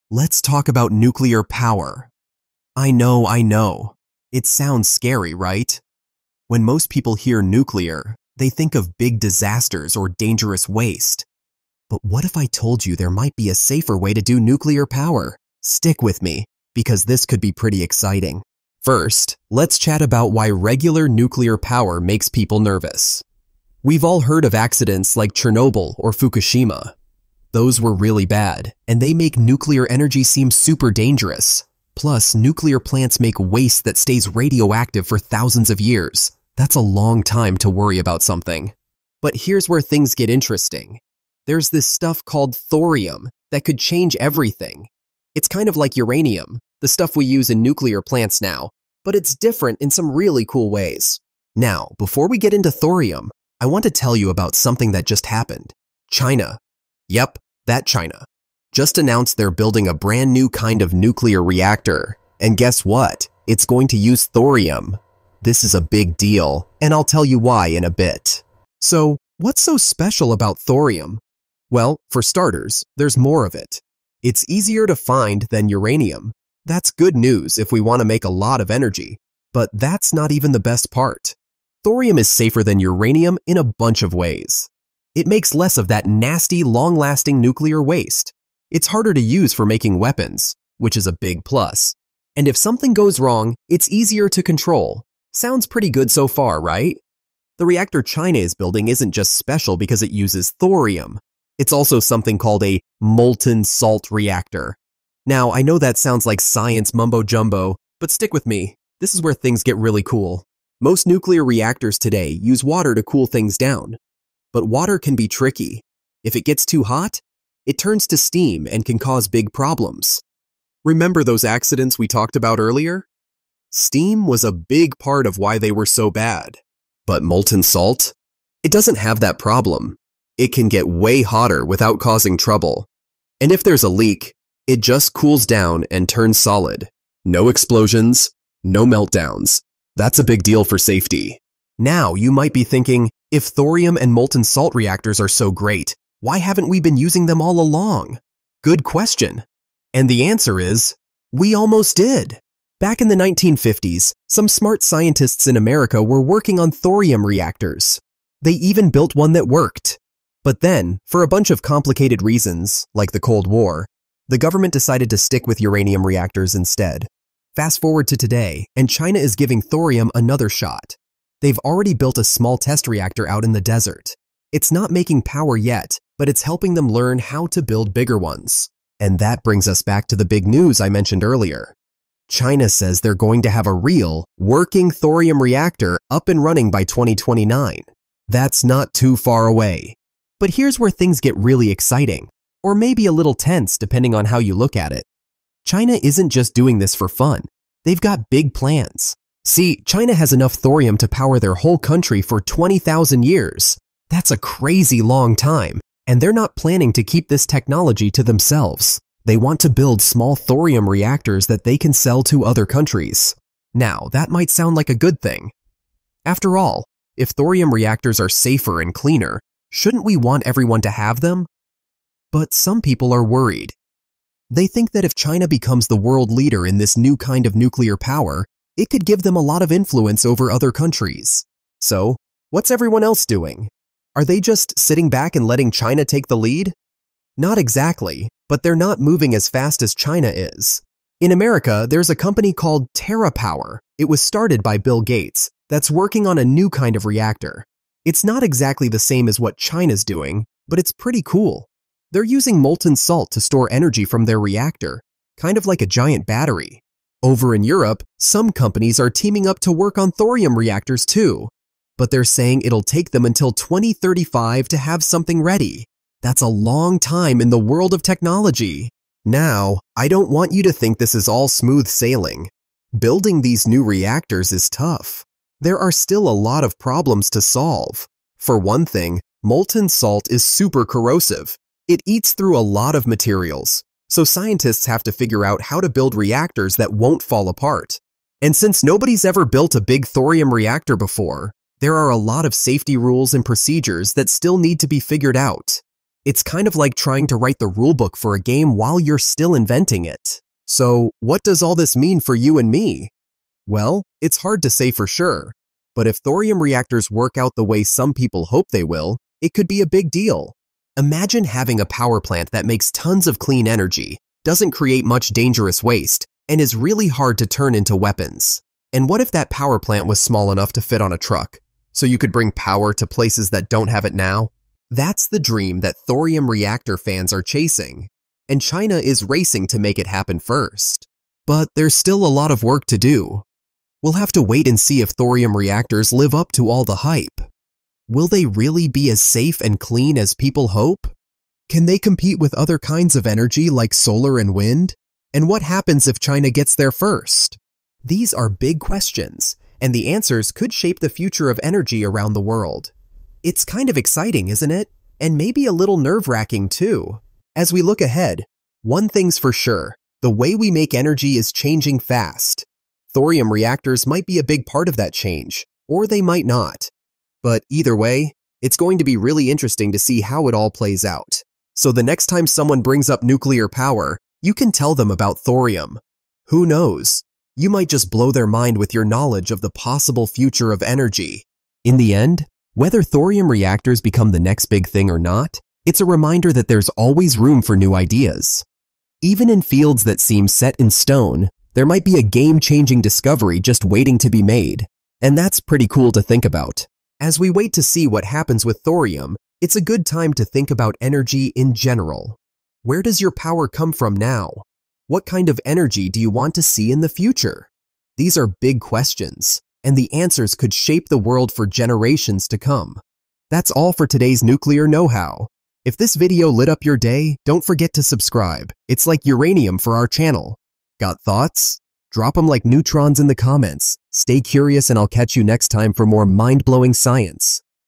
China's Secret Weapon The Thorium Reactor That Could Change Everything 2024
Nuclear Power's Next Frontier: China's Thorium Gambit
China unveils plans for the world's first molten salt thorium reactor. Safer? Cleaner? Revolutionary? Discover how this ambitious project could reshape global energy. Is thorium the key to our nuclear future?